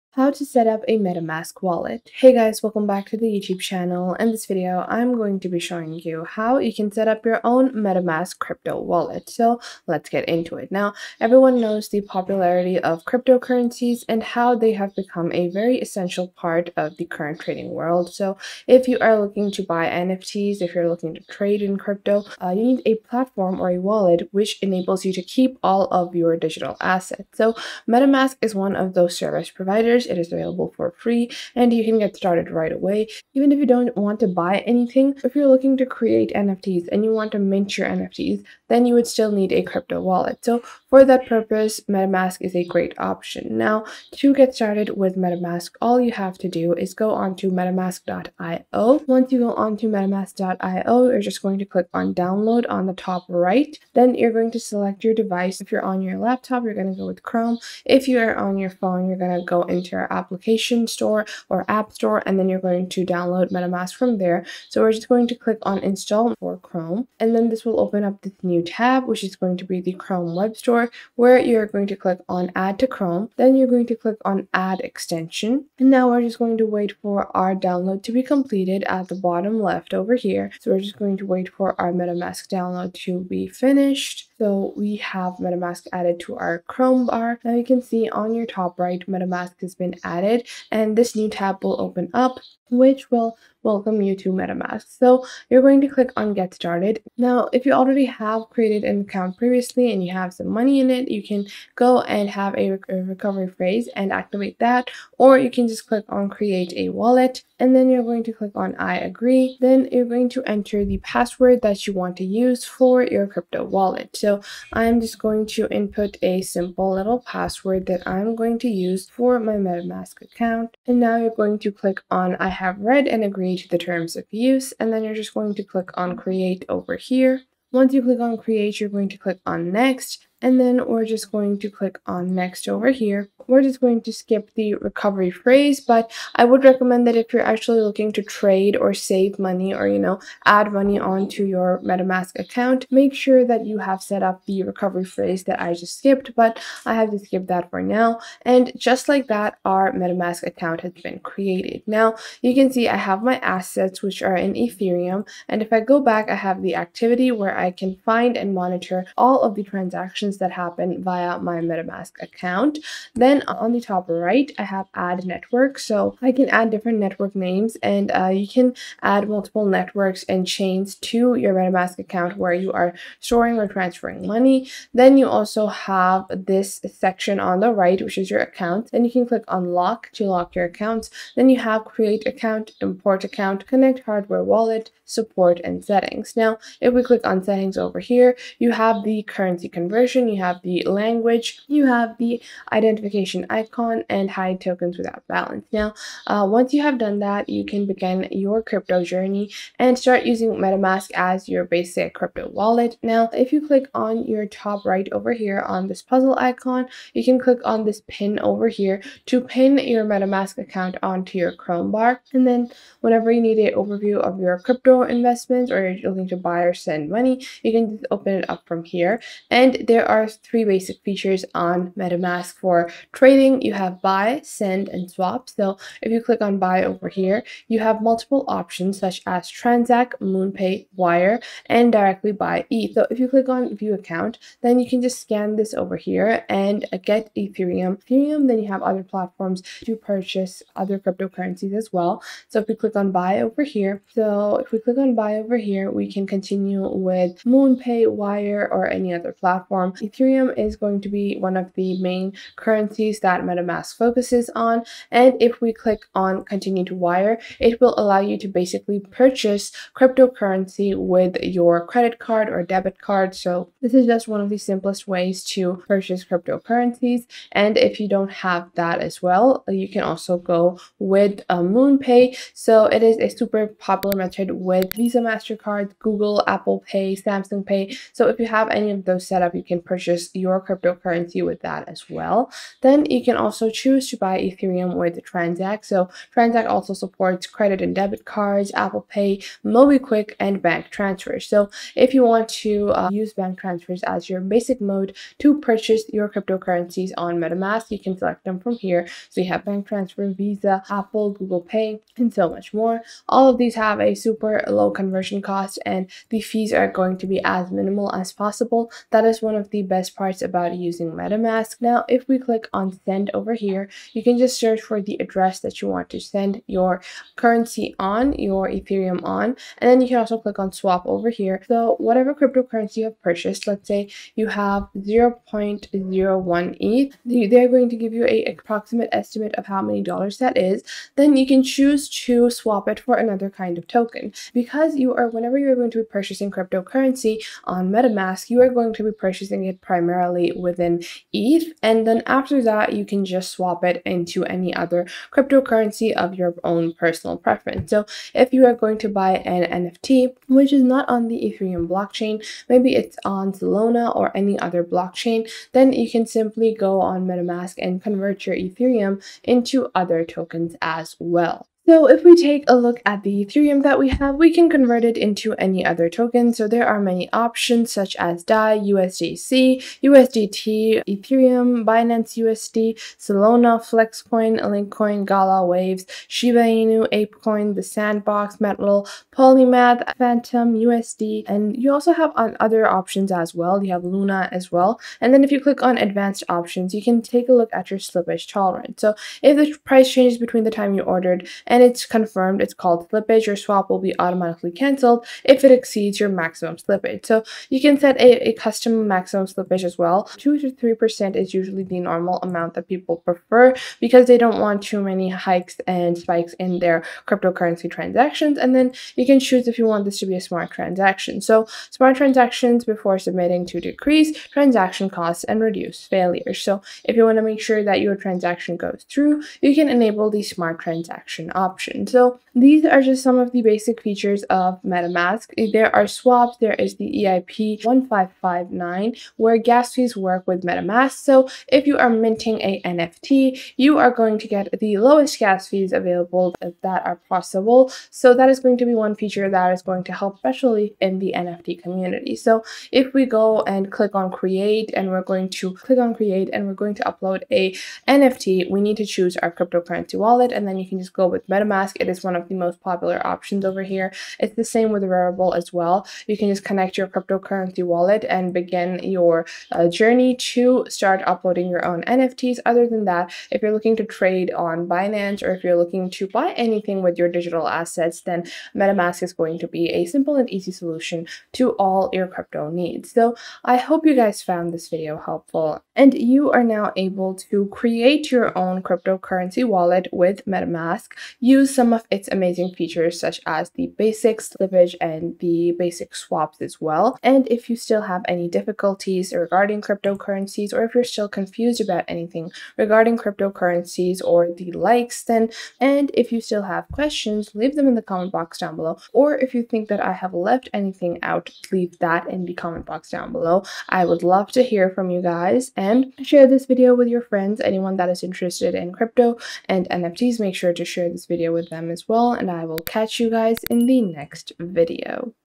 How To Setup A Metamask Crypto Wallet | Tutorial (2024)
In this video I show you How To Setup A Metamask Crypto Wallet. This is super easy and learn to do it in just a few minutes by following this helpful tutorial.